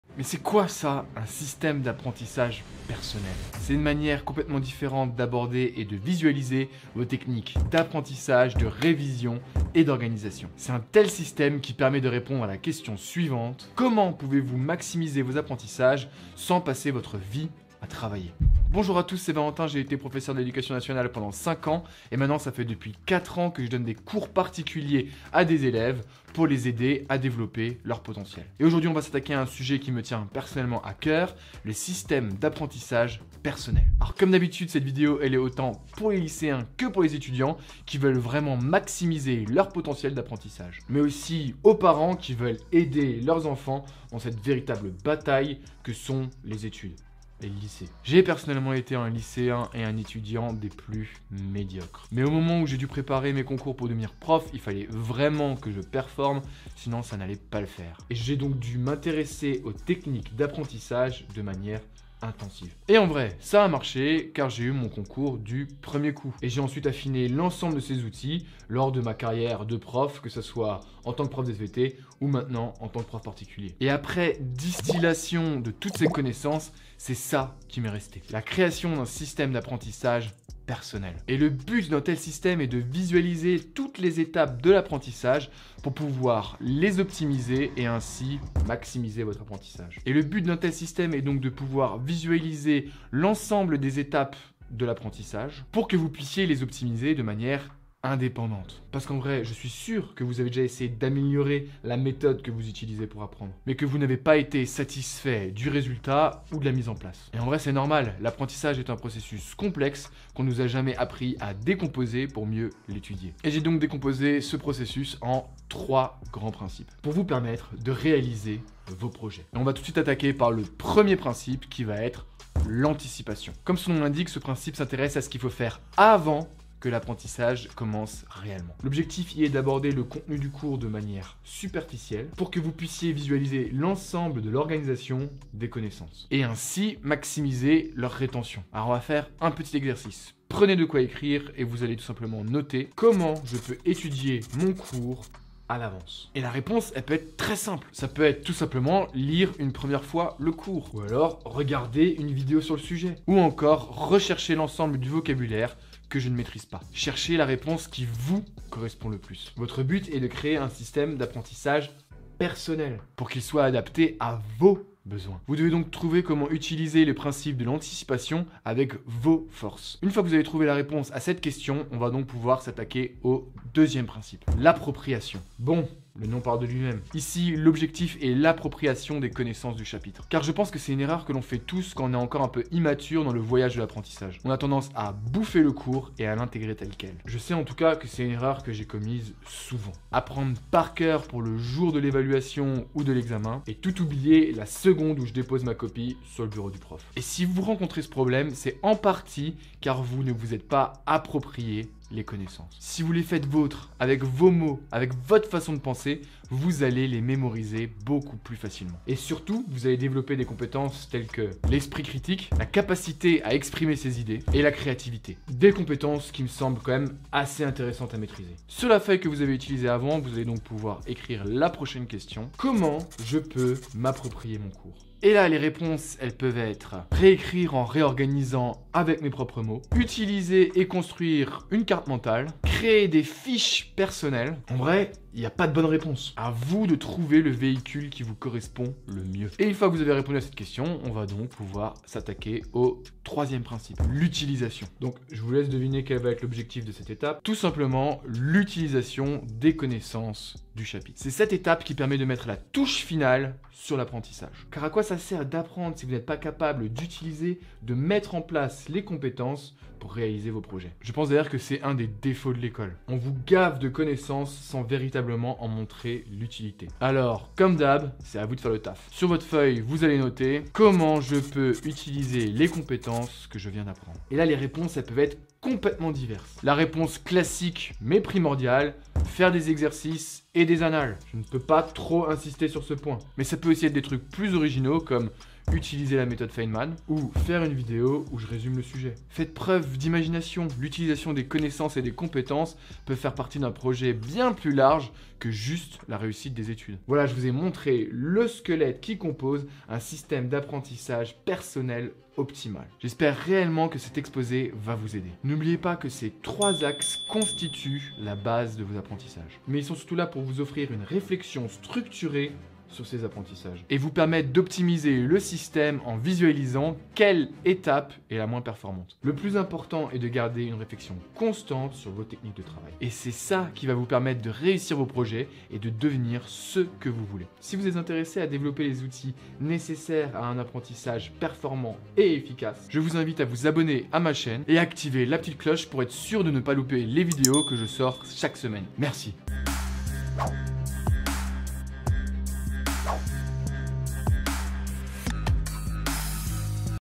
Qu’est-ce qu’un SYSTÈME D'APPRENTISSAGE PERSONNEL ?!
Cette méthode change tout ! Elle vous offre une autre manière d’aborder vos apprentissages !